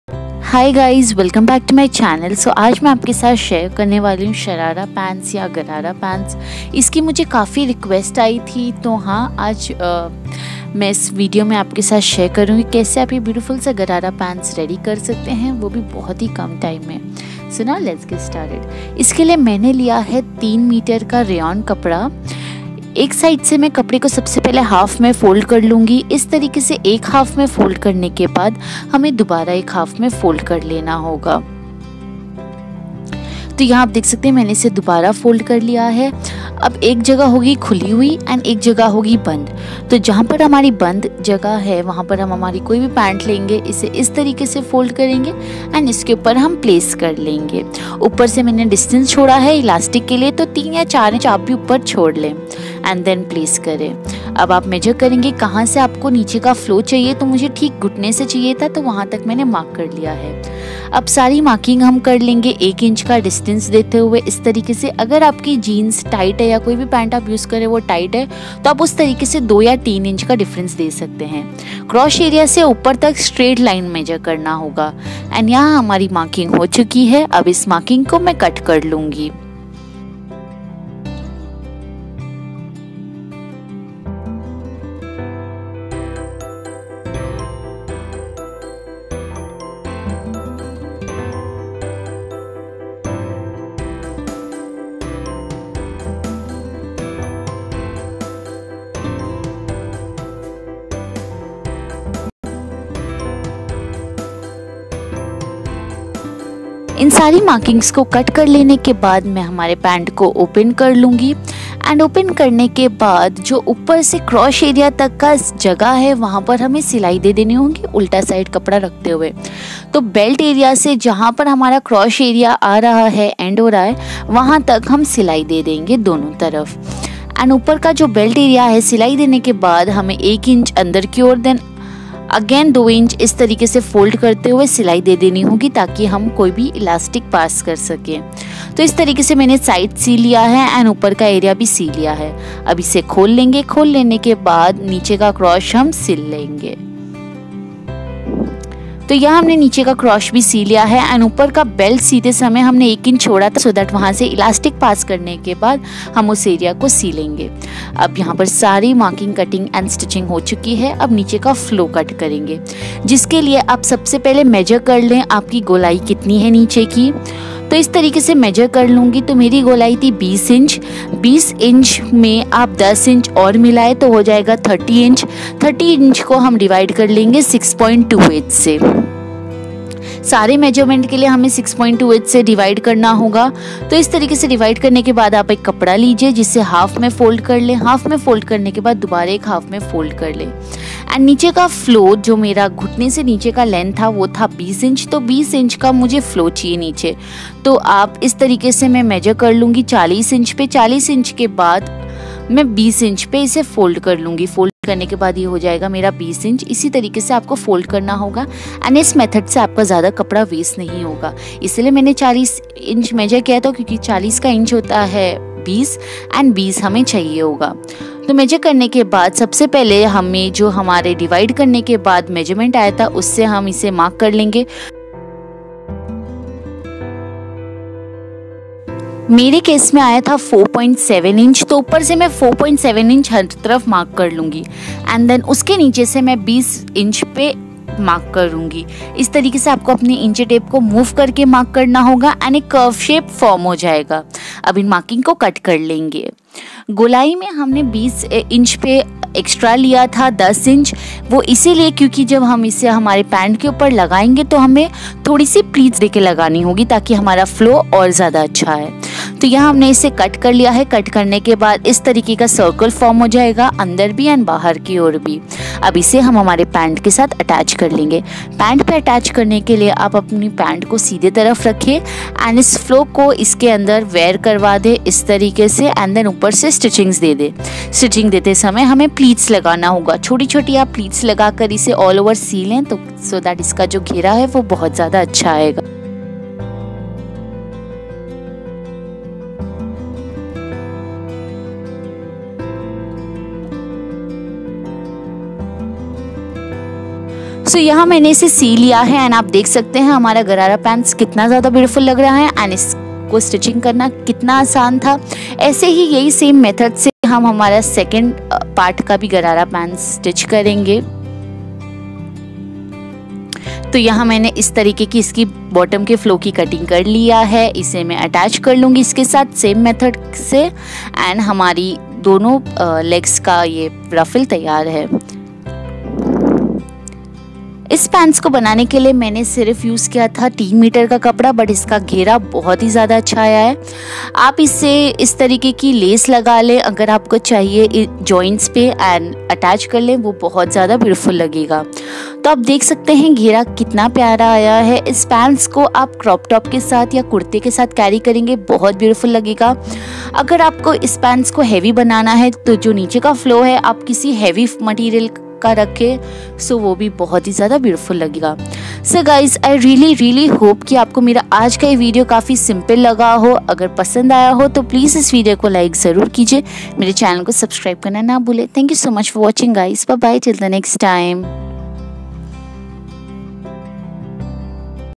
DIY : Gharara/Sharara Cutting and Stitching || Gharara EASY making||Latest Sharara Dress Design
Gharara/Sharara are amazing because they’re so incredibly comfortable. With the right styling, they can work for cold weather, but they’re really ideal for warmer weather because they’re so light. Surprisingly, Sharara are also very versatile, Today i am sharing a stylish Garara/Sharara diy..i hope you guys like my
 efforts :)